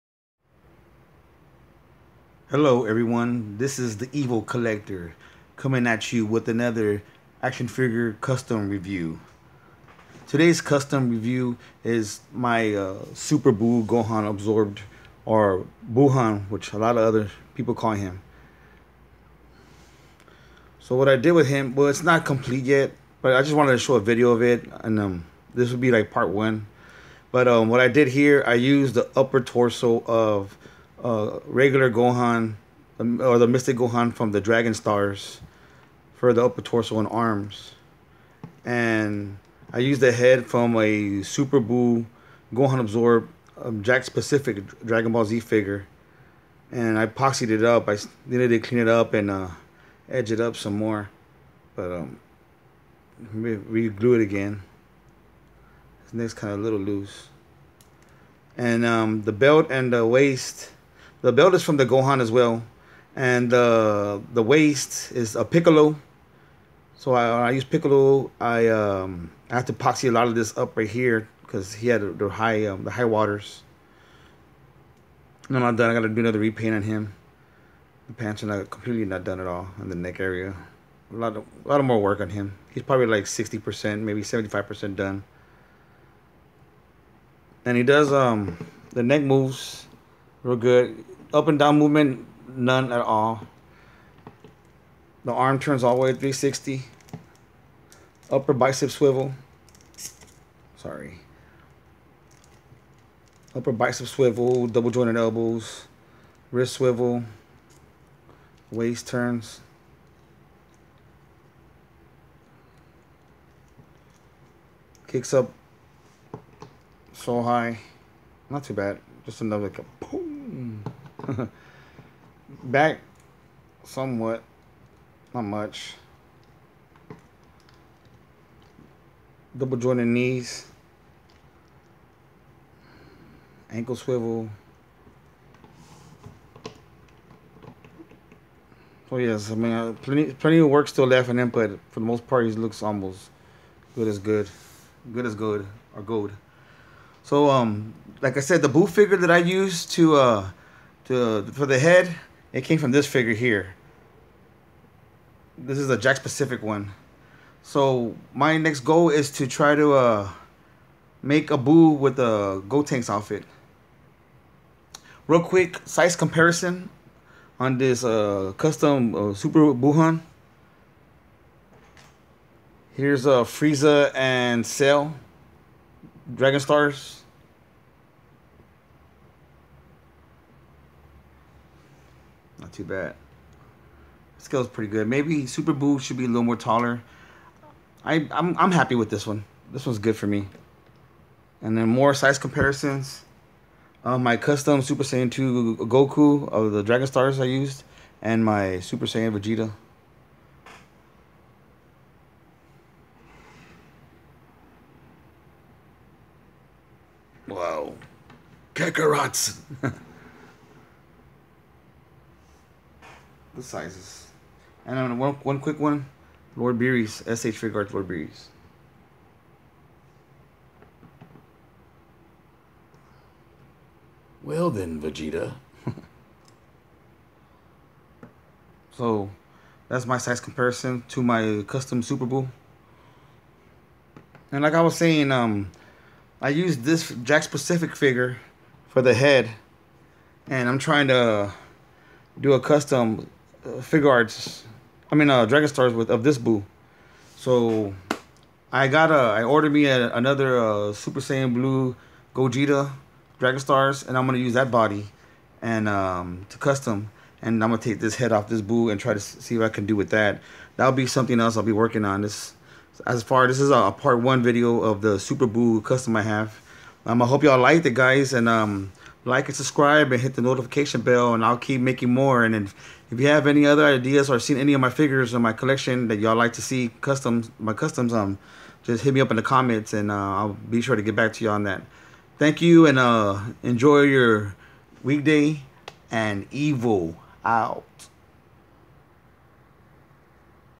Super Buu (Buuhan) custom figure review pt.1
Action figure review of a custom dragon stars Super Buu figure